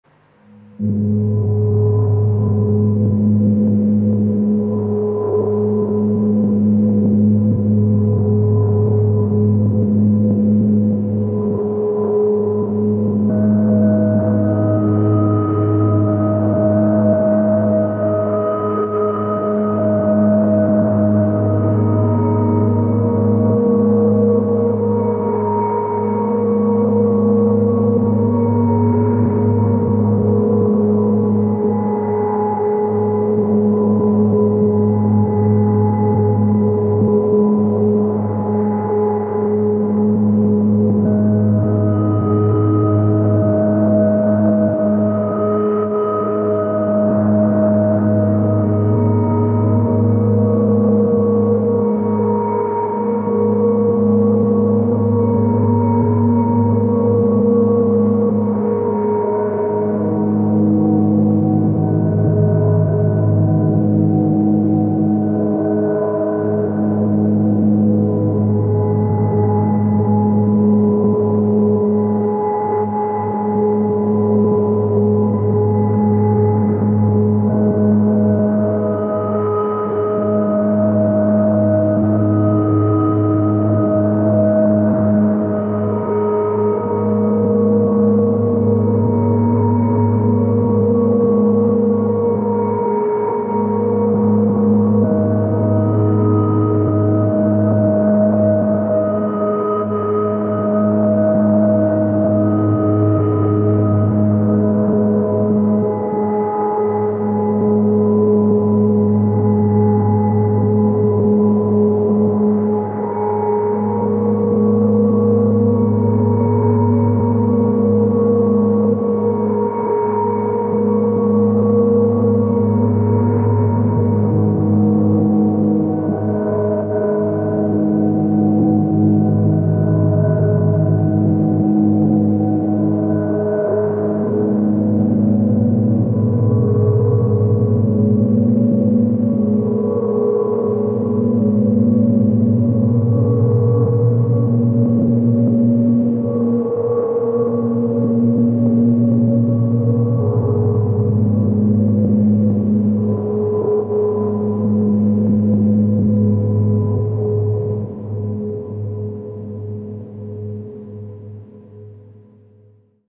Microfreak and pedals
It is now possible to import own sounds to the Microfreak. Very useful.